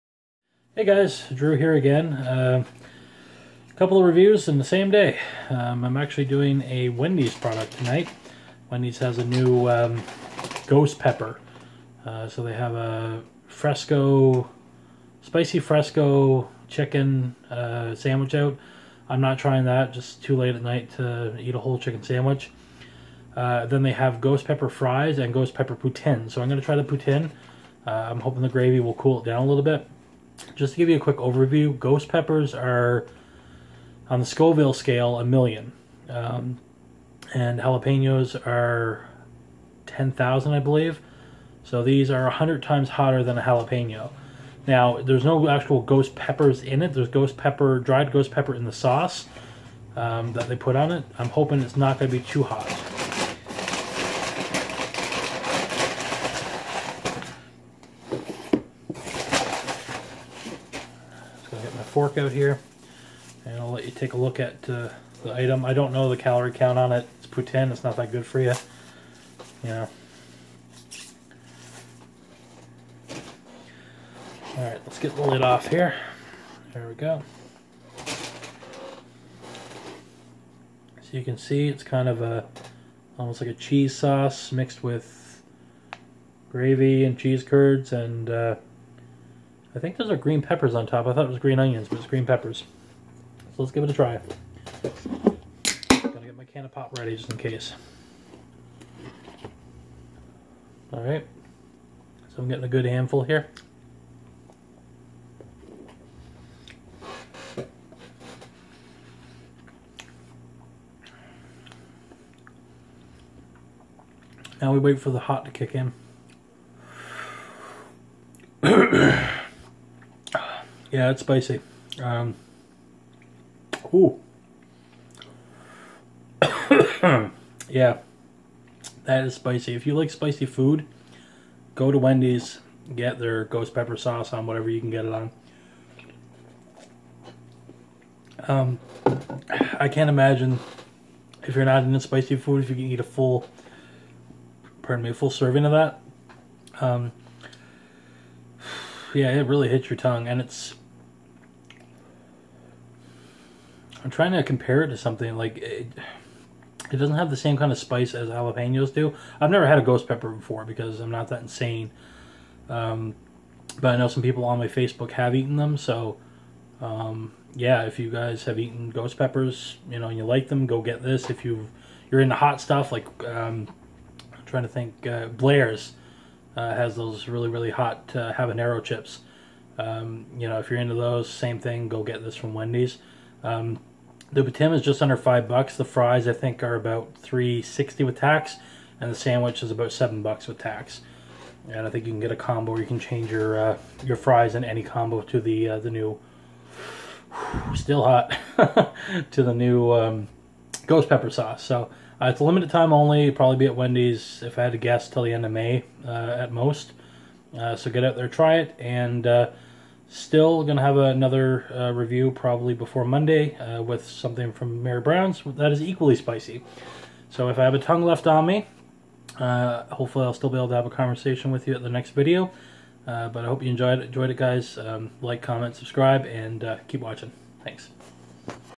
Drew Reviews Wendy's Ghost Pepper Poutine
The heat is on and Drew is taking one for the team and trying out Wendy's new Ghost Pepper Sauce. Will he make it out alive?!